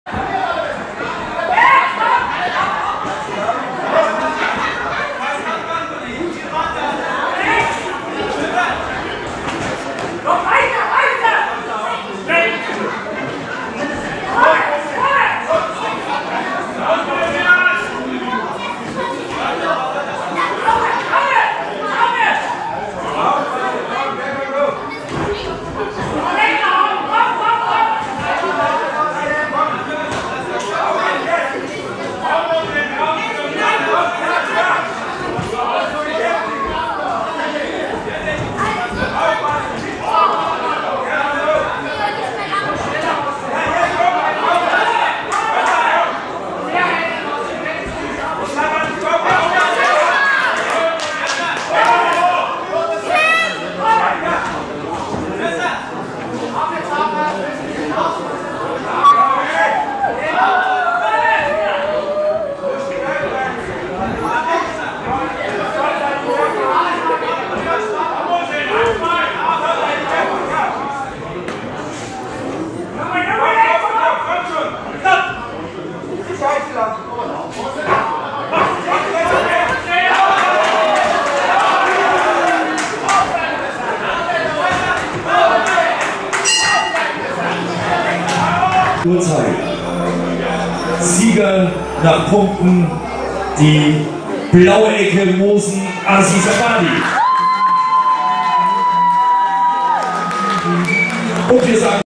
BM2018 - K03 Cem vs Azizabadi (-60Kg)
Bayerische Box-Meisterschaft am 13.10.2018 in Straubing. Azizabadi gewinnt im Leichtgewicht -60Kg über Yilmaz Cem nach Punkte.